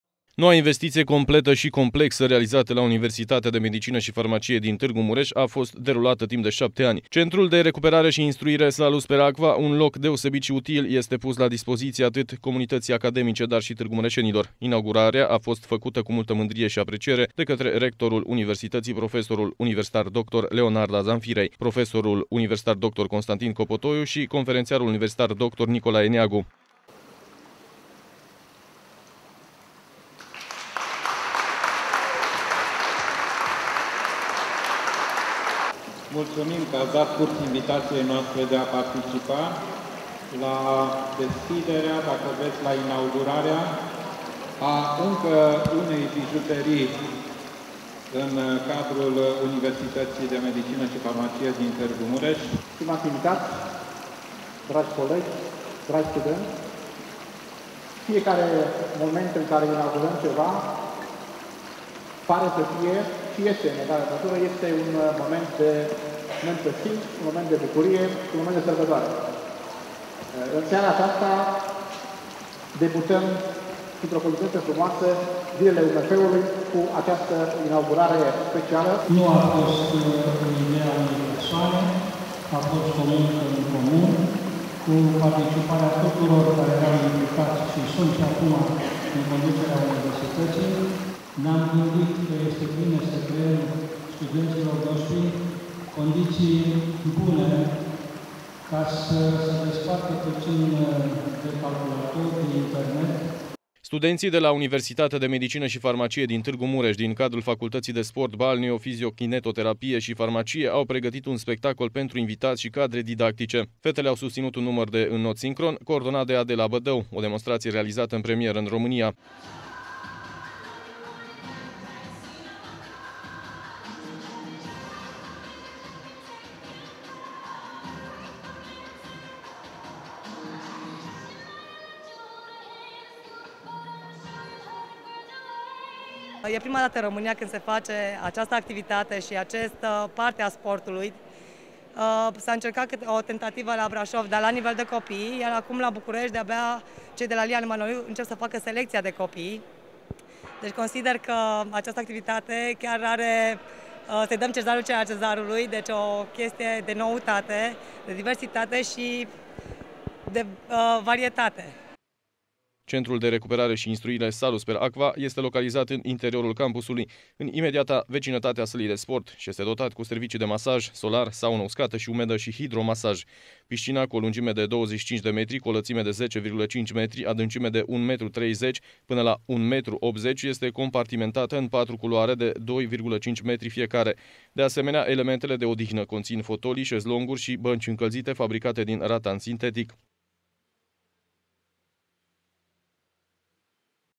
SALUS PER AQUA, NOUA BIJUTERIE UMF
Odată cu debutul Zilelor Universităţii de Medicină şi Farmacie din Tîrgu Mureş, Universitatea a inaugurat Centrul de Recuperare şi Instruire Salus per Aqua, ultima investiţie de amploare finalizată de instituţia de învăţământ superior. Noul obiectiv oferă o multitudine de facilităţi de instruire, dar şi de relaxare pentru studenţi şi cadrele didactice.